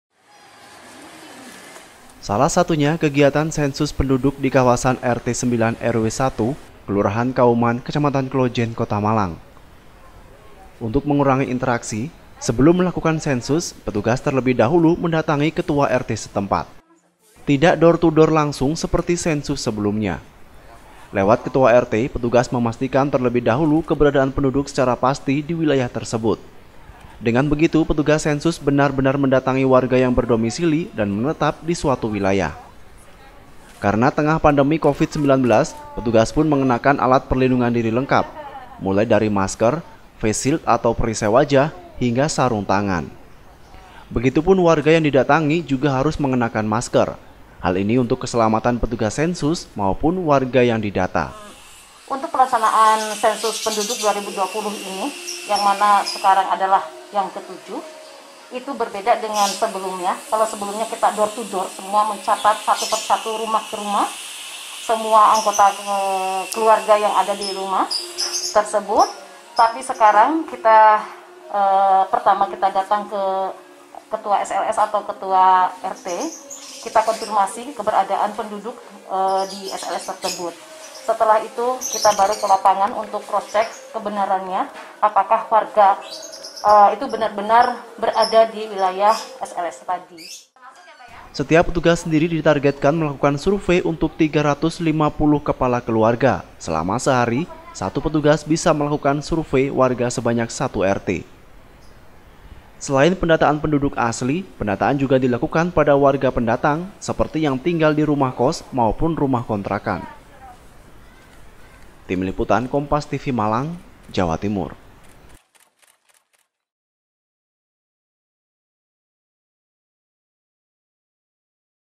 Sensus Penduduk di Tengah Pandemi
MALANG-KOMPAS.TV-Sensus penduduk yang digelar Badan Pusat Statistik (BPS) tahun ini berbeda lantaran dilakukan di tengah pandemi.

Seperti sensus penduduk di kawasan RT 9 RW 1 Kelurahan Kauman Kecamatan Klojen Kota Malang.

Untuk mengurangi interaksi, sebelum melakukan sensus petugas terlebih dahulu mendatangi ketua RT setempat.

Tidak door to door langsung seperti sensus sebelumnya.

Lewat ketua RT setempat, petugas memastikan terlebih dahulu keberadaan penduduk secara pasti di wilayah tersebut.

Selain itu petugas juga mengenakan APD lengkap.

"Karena kondisi pandemi, kami harus memastikan memakai alat pelindung diri demi keselamatan diri sendiri dan warga yang kita survei," ujar Elismawati, salah seorang petugas sensus, Kamis (3/9/2020).

Setiap petugas sensus ditargetkan melakukan survei untuk 350 KK.

Dalam sehari satu petugas bisa melakukan survei sebanyak satu RT.